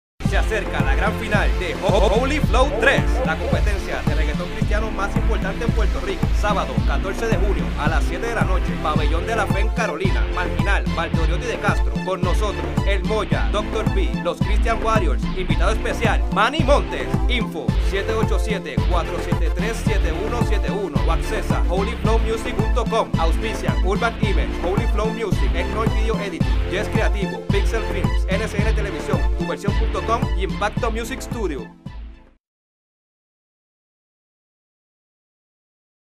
GRAN Final HF 3
Promo de TV para el evento final de Holy Flow Reggaeton Contest 2008